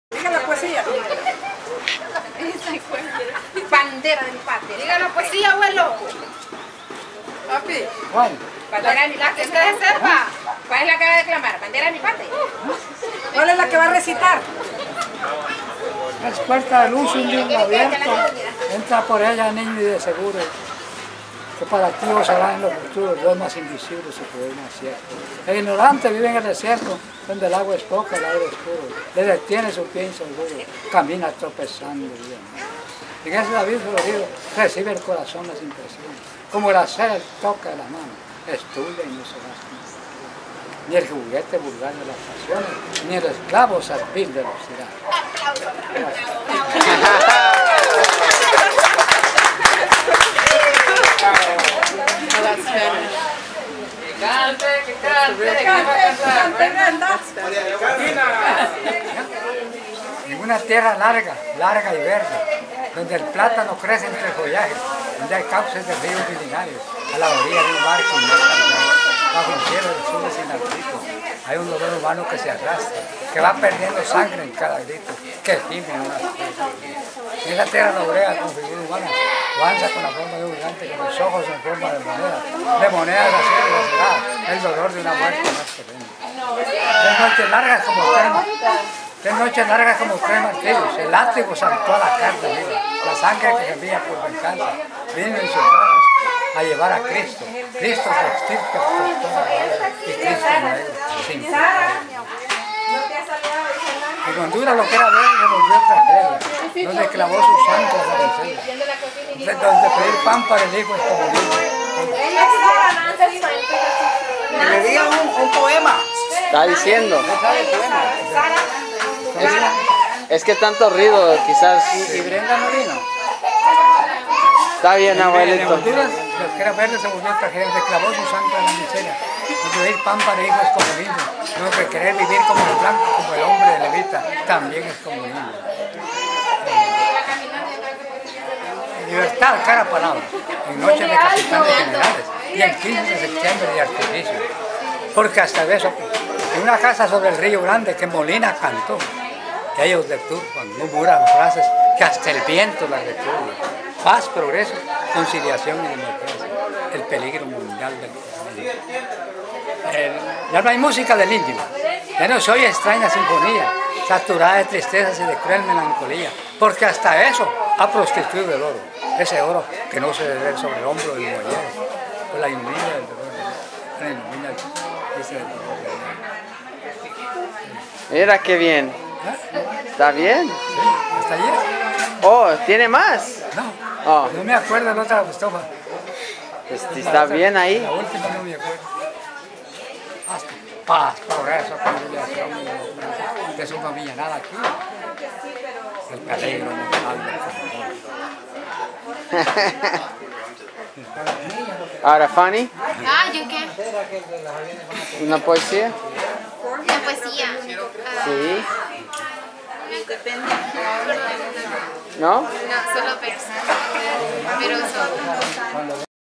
Abuelito Robles
Video of Abuelito Robles saying a few poems.  Taken in 2006 when he was 93 years old.  What a great memory and ability!